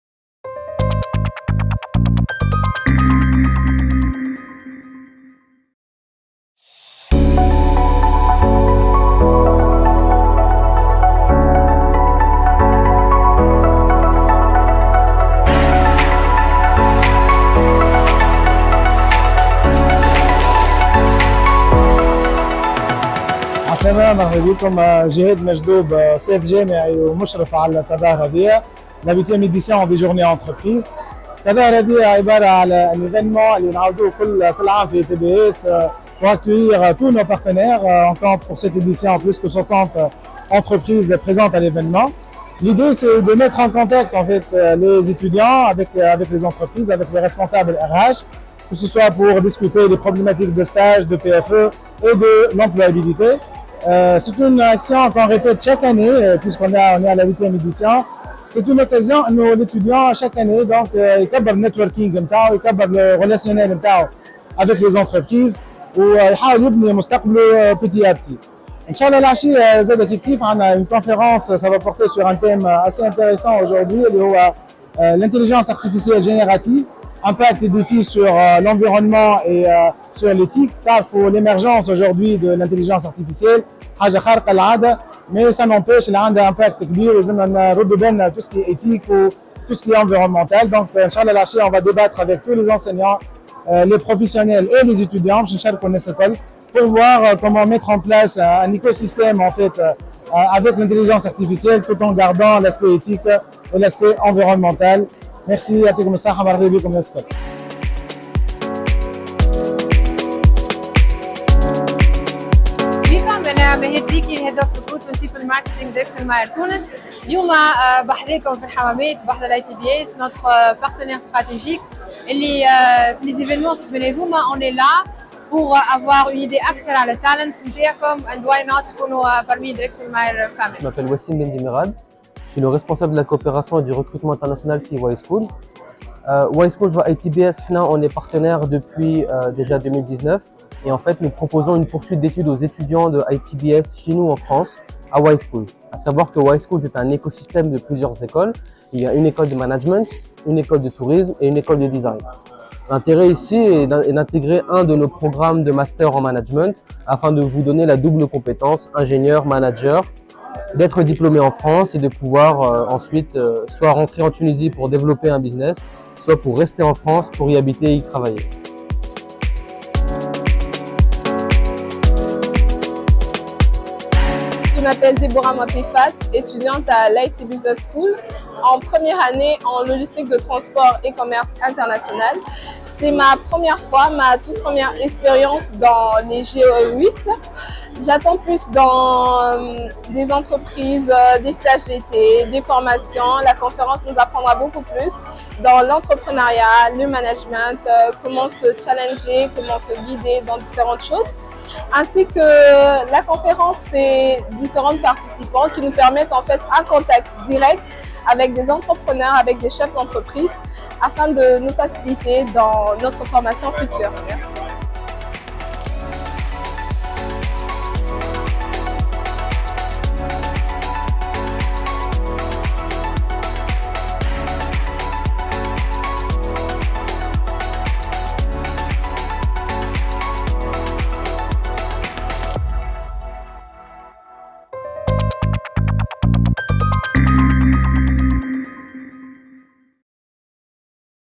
8ème édition de JEE organisée par l'ITBS le 20 et 21 Avril 2024
Un retour en vidéo sur la couverture de la 8ème édition de JEE organisée par l'ITBS le 20 et 21 Avril 2024 sous le thème : 𝐉𝐄𝐄𝟎𝟖 : 𝐆𝐞𝐧𝐞𝐫𝐚𝐭𝐢𝐯𝐞 𝐀𝐈 - 𝐄𝐭𝐡𝐢𝐜𝐬 𝐂𝐡𝐚𝐥𝐥𝐞𝐧𝐠𝐞 𝐚𝐧𝐝 𝐄𝐧𝐯𝐢𝐫𝐨𝐧𝐦𝐞𝐧𝐭𝐚𝐥 𝐈𝐦𝐩𝐚𝐜𝐭
Avec la participation de +800 étudiants, diplômés, enseignants et professionnels, 𝐥𝐞𝐬 𝐉𝐨𝐮𝐫𝐧𝐞́𝐞𝐬 𝐄𝐧𝐭𝐫𝐞𝐩𝐫𝐢𝐬𝐞𝐬 𝐈𝐓𝐁𝐒 est une occasion de rencontre et d'échange sur différentes niveaux, tel que les stages, l'employabilité ainsi que la thématique de l'évènement 🚀⏳